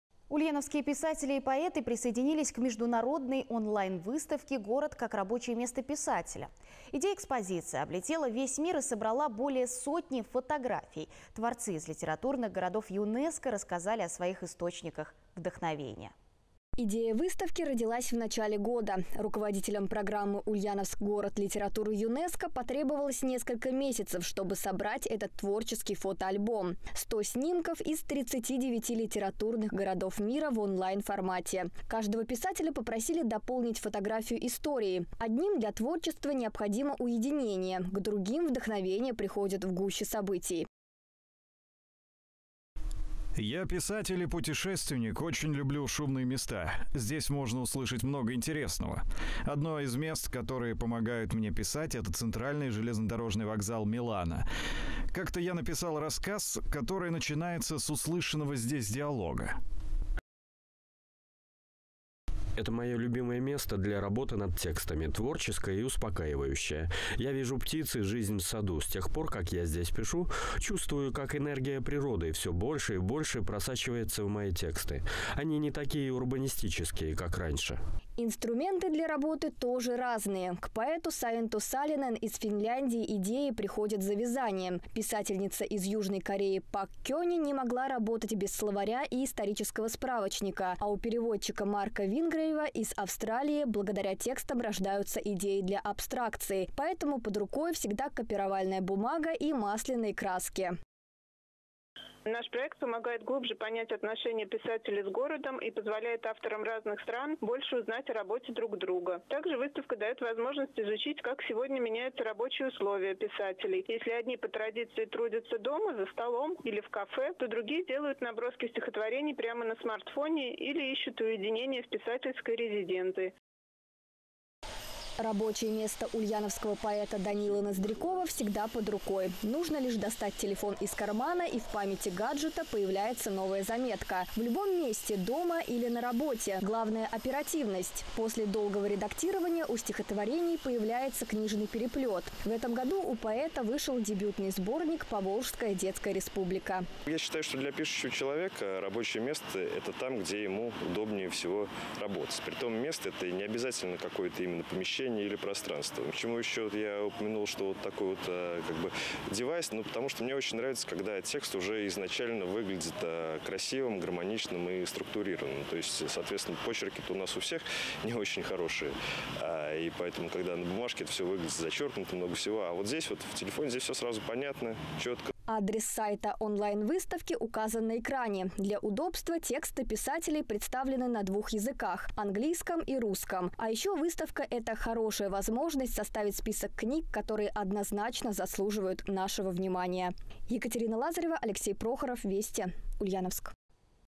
«Город как рабочее место писателя»
Ульяновские писатели и поэты присоединились к международной онлайн-выставке «Город как рабочее место писателя». Идея экспозиции облетела весь мир и собрала более сотни фотографий. Творцы из литературных городов ЮНЕСКО рассказали о своих источниках вдохновения.